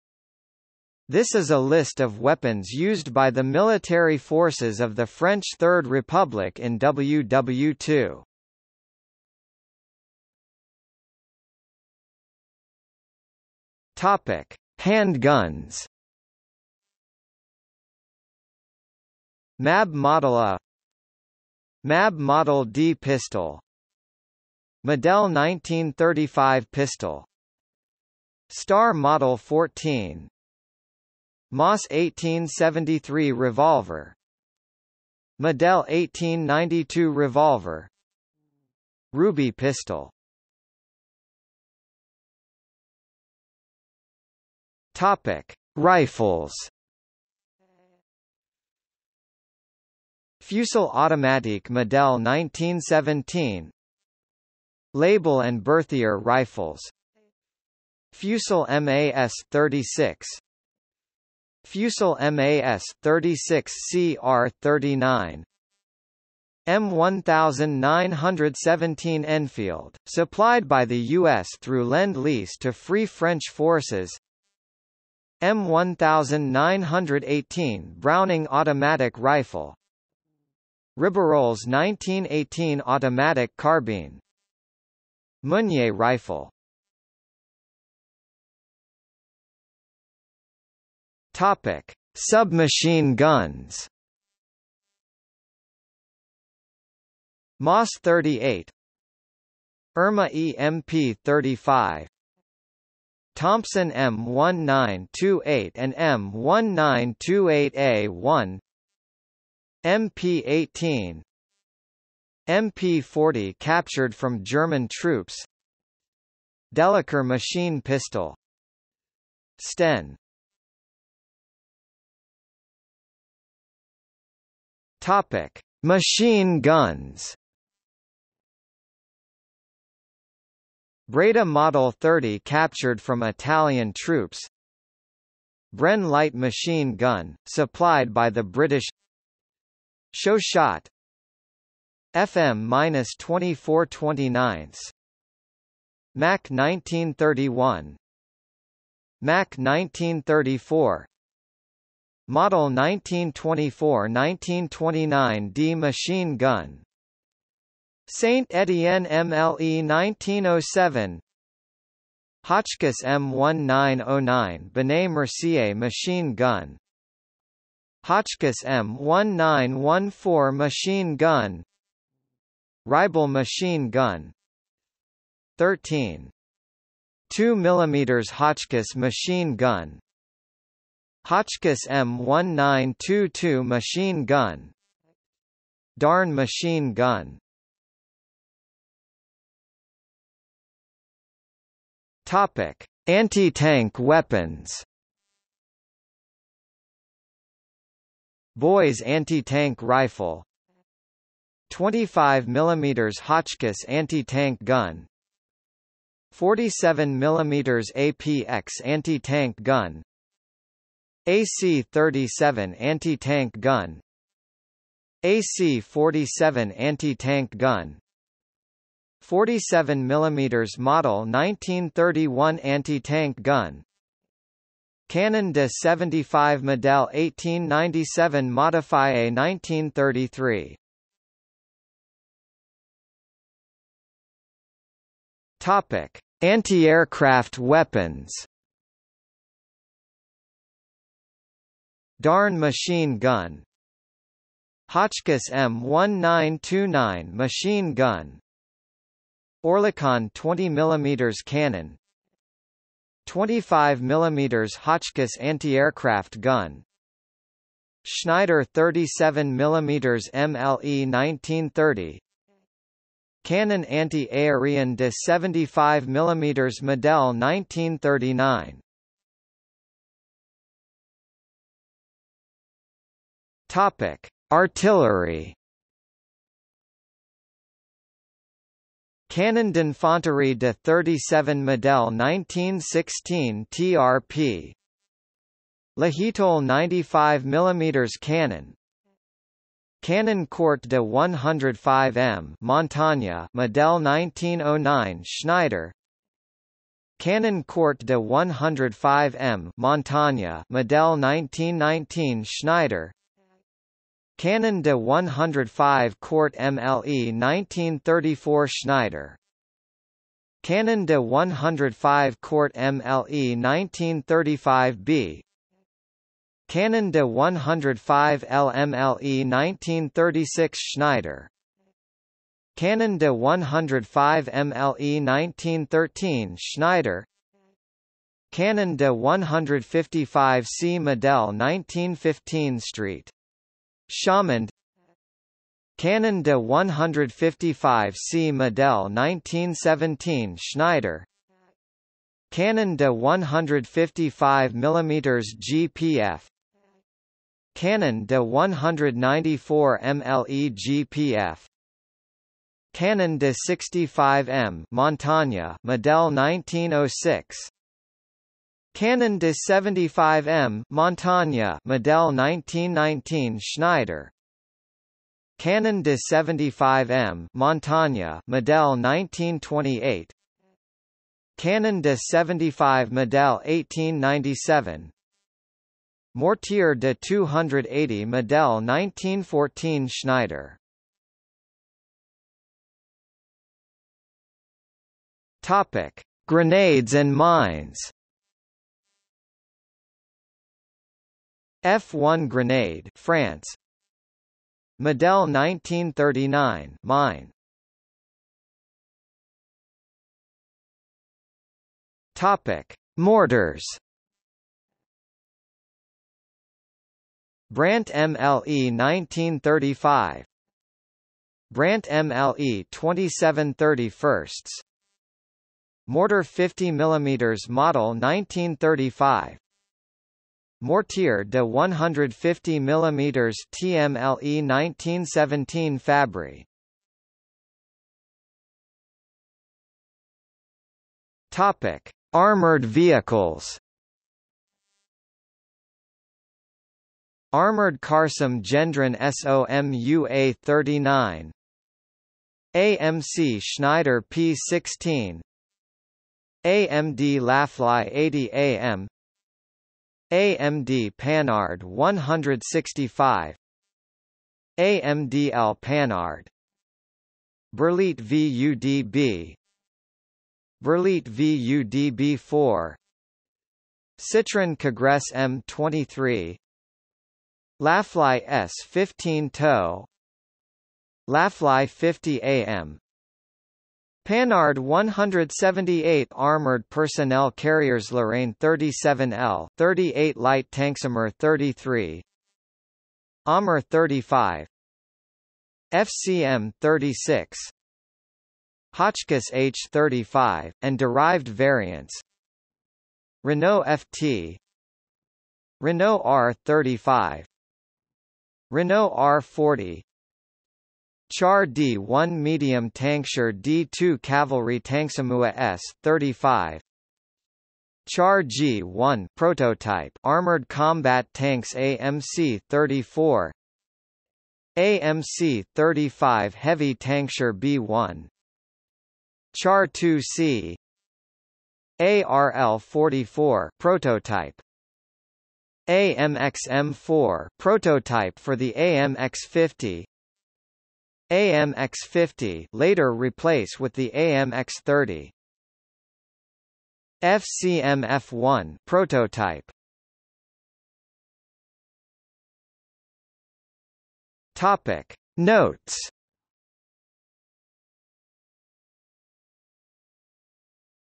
List of World War II weapons of France | Wikipedia audio article
00:00:10 1 Handguns
00:00:41 2 Rifles
00:01:27 3 Submachine guns
00:02:01 4 Machine guns
00:03:08 5 Anti-Tank weapons
00:03:51 6 Anti-Aircraft weapons
00:04:30 7 Artillery
00:06:22 8 Grenades and mines
00:06:38 9 Mortars
00:07:07 10 Armoured vehicles
00:09:00 11 Notes

- Socrates

SUMMARY
This is a list of weapons used by the military forces of the French Third Republic in WW2.